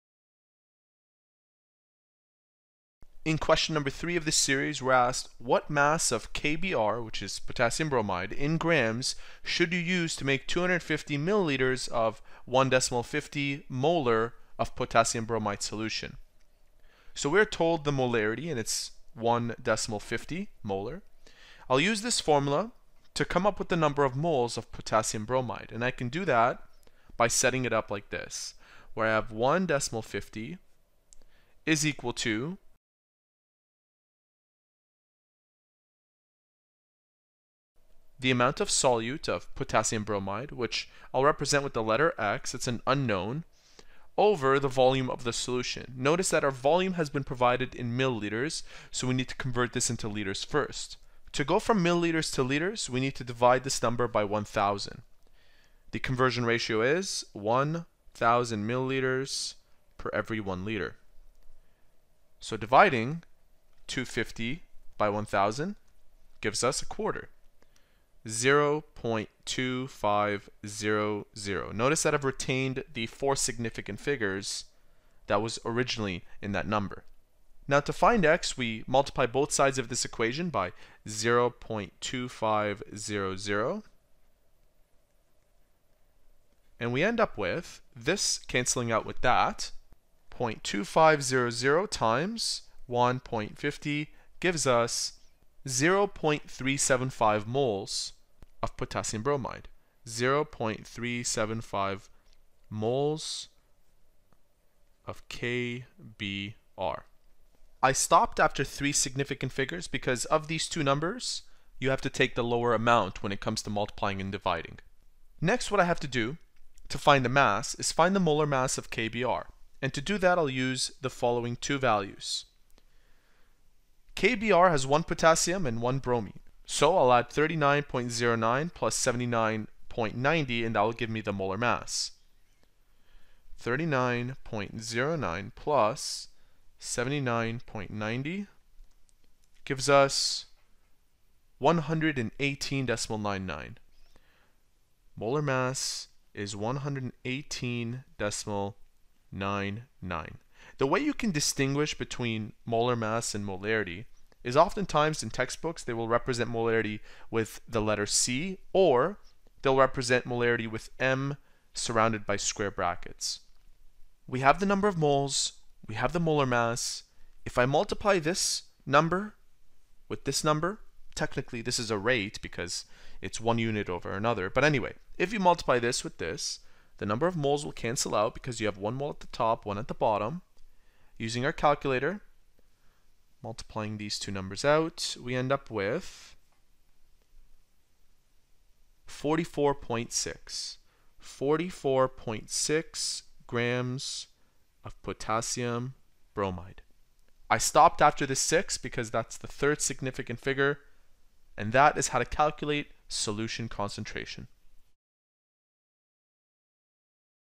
⚗️ Calculating Solution Concentration (Question 3)
🤔 Still stuck in math? to start asking questions.

What mass of KBr (in grams) should you use to make 250.0 mL of a 1.50 M KBr solution?

Molarity (M):   ((amount of solute (in mol))/(volume of solution (in L)))

Molar mass of potassium (K):   (39.09 g/mol)

Molar mass of bromine (Br):   (79.90 g/mol)